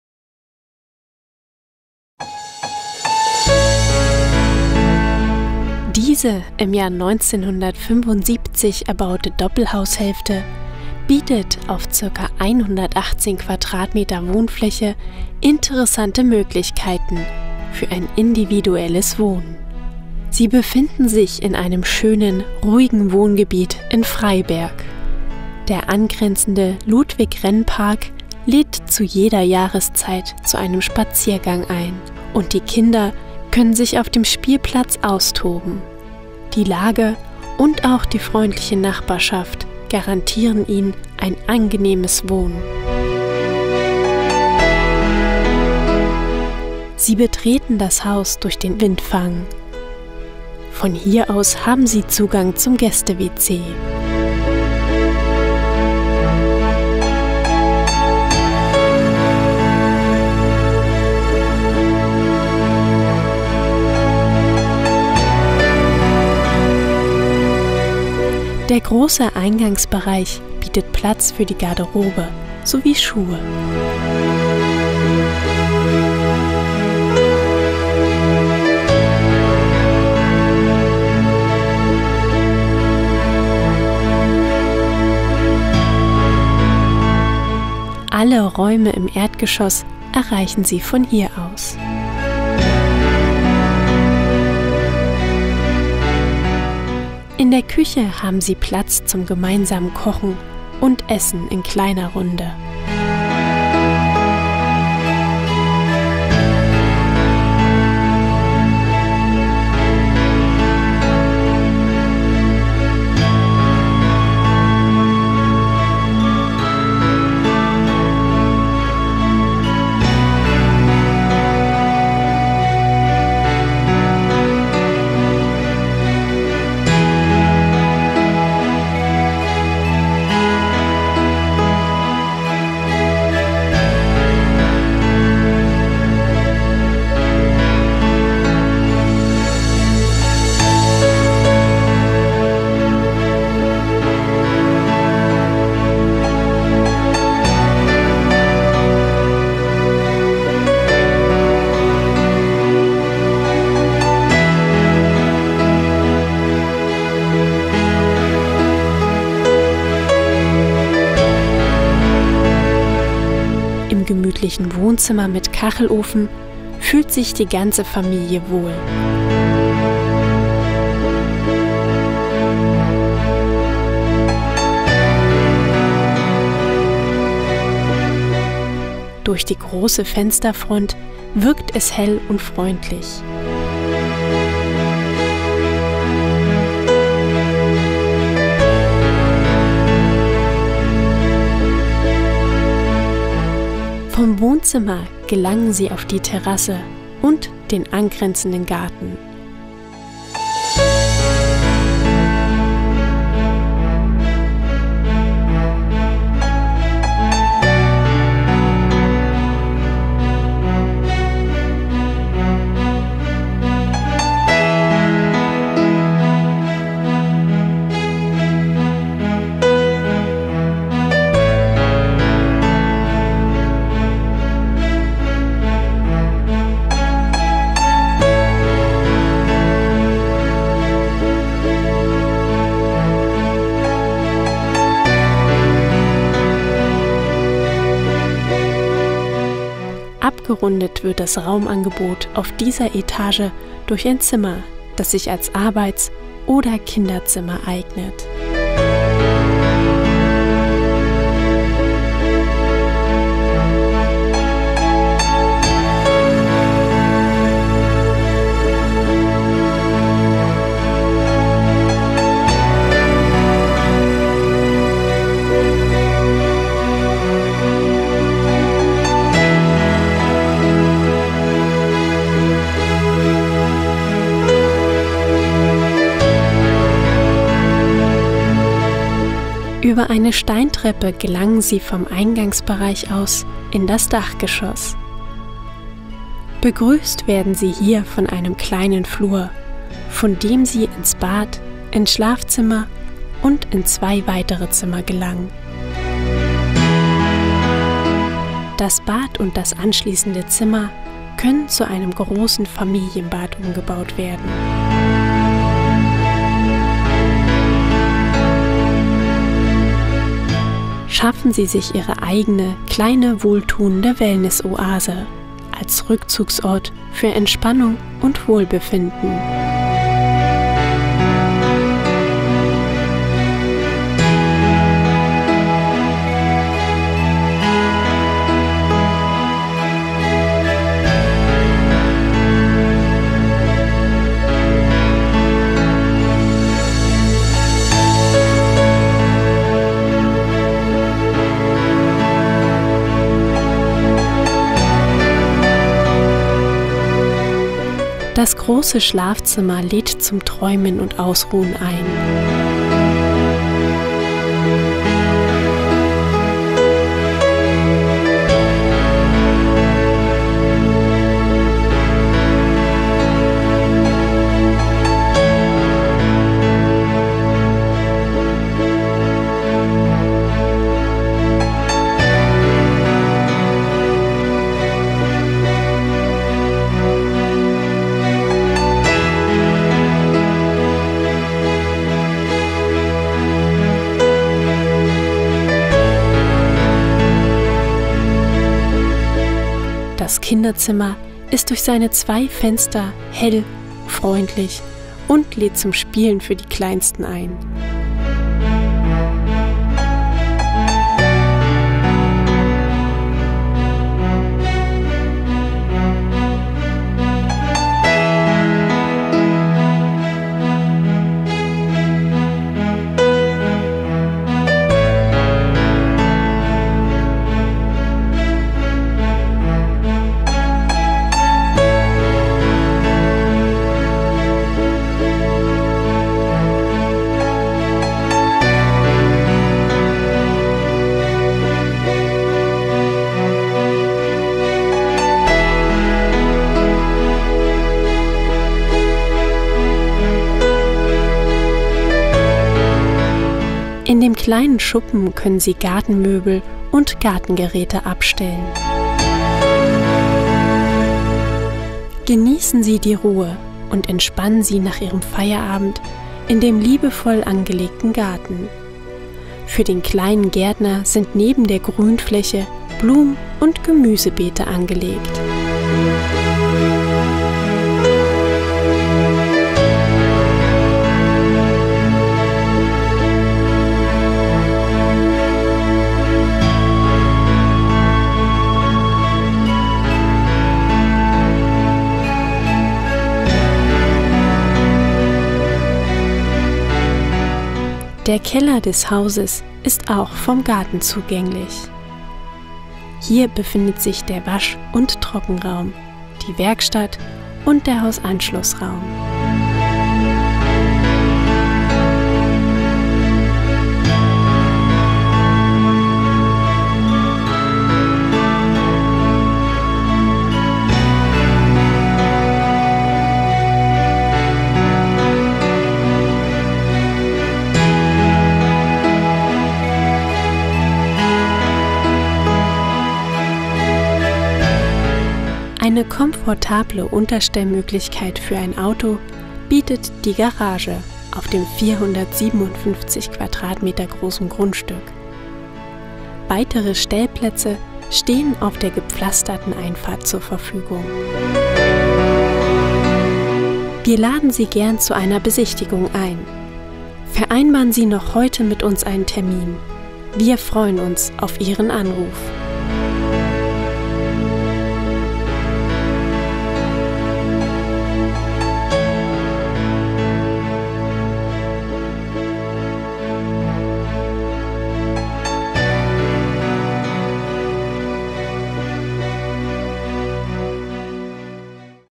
Doppelhaushälfte zu verkaufen | Freiberg
Ort: Freiberg
Objektart: Doppelhaushälfte
Grundstücksfläche: 457 m²
Wohnfläche: 118 m²
Kaufpreis: EUR 151,000

HINWEIS:
IMMOBILIE IST BEREITS VERKAUFT !